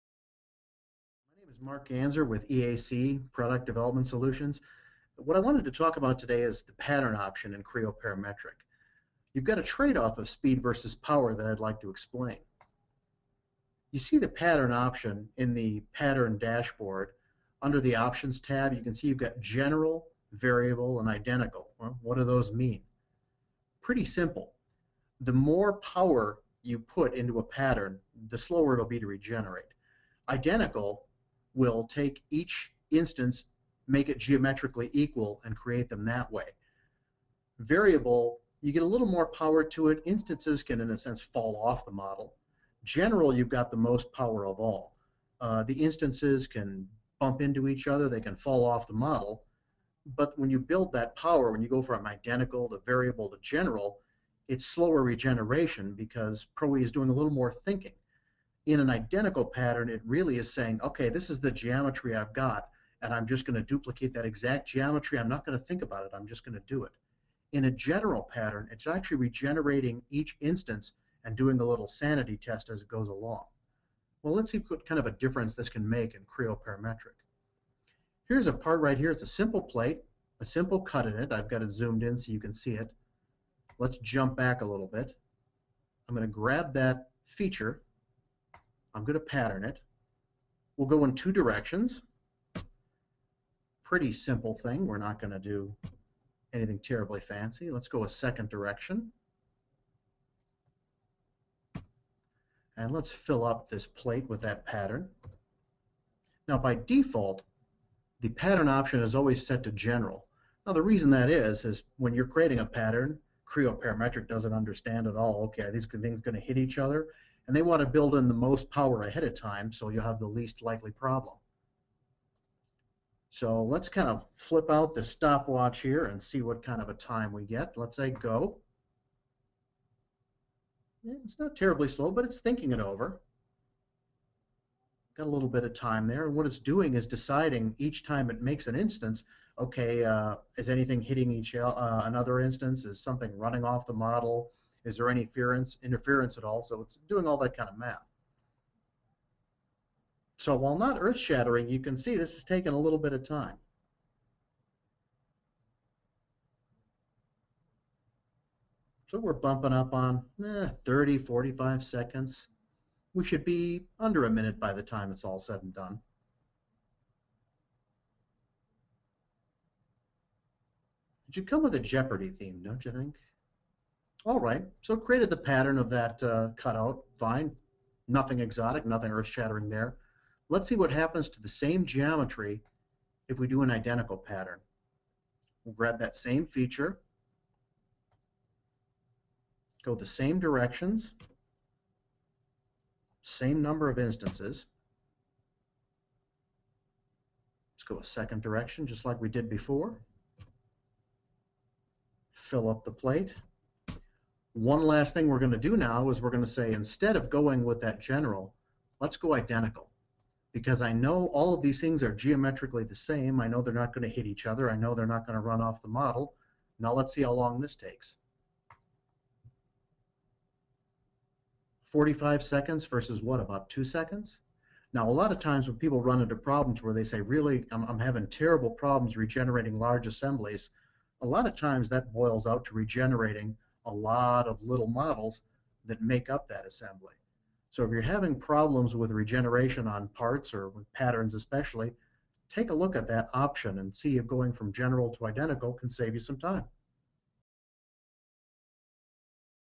Creo Pattern Options - Speed vs. Power: EAC Tip of the Week
During this video you will see how to save a lot of processing time simply by changing the pattern type option in Creo Parametric and Pro/ENGINEER.

In Creo Parametric and Pro/ENGINEER there are three different pattern types that render in three very different ways. Some ways can render a pattern in less than 10% of the time it takes for other types. If you have performance issues when rendering assemblies, parts, or patterns, then this might be an easy fix.